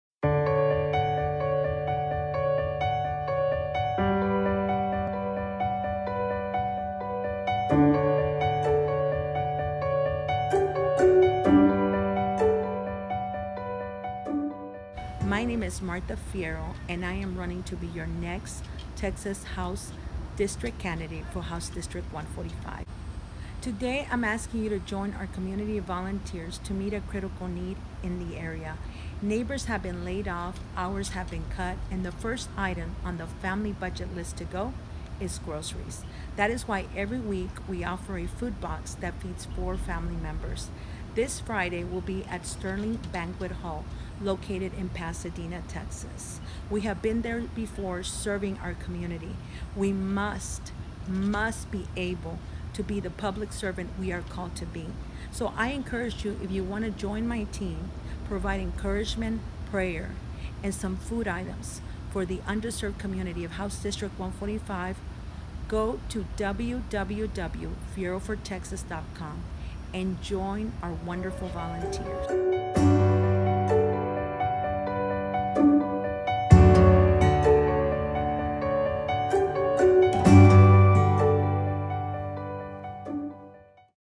Former Nurse Gathers Volunteers to Provide Food and Support Houstonians
Over 2,000 families in need to be served by over 40 community volunteers

Houston, Texas (August 20, 2020) – The Farms to Families initiative under the Cares Act has enabled community leaders to provide prepared food boxes valued at $50 which feeds a family of 4 with items like potatoes, cantaloupe, red/green apples, carrots, and more to people across America.

Many may recall that farmers were being forced to throw away crops and milk due to the Covid-19 shutdown that disturbed food distribution and forced owners to destroy their products at a critical time.

One Texas State Representative Candidate Martha Fierro has rallied people in her district HD145, which runs along the I-45 Corridor in an area critically underserved.

“I am a retired nurse and now minister, who has invested in my community, and I realized that I could rally those in my network to make a difference during this time by working with area churches and others who wanted to help families who desperately needed these food items as well as support and encouragement,” said Marth Fierro, “To date, we have provided food, prayer, and our volunteers and companies have provided other resources to reach over 8,000 families in dire need.”

This initiative will be ongoing every week in various locations, with an expected turn-out for this offering with over 2,000 recipients expected.

Date:  Friday, August 21, 2020
Time:  7pm for food recipients/5:30pm for volunteers
Location:  Sterling Banquet Hall – 116 Fairmont Parkway, Pasadena, Texas 77504
• Free masks will be provided to those who attend

"During this time, there is a great need for many and At His Feet Ministries can never sit by and watch people in distress when we can do the best we can to bring relief," said Pastor Richard Vega, Outreach Pastor of
CT Church Houston (one of the participating area churches).

"This event brings life back into the communities. The prayer and the encouragement 'Dale Fierro' means to ‘Keep going!’ This brown box feeds our families and it shows that Martha Fierro is one of us and cares for us," said Monica Garcia (Houston area recipient).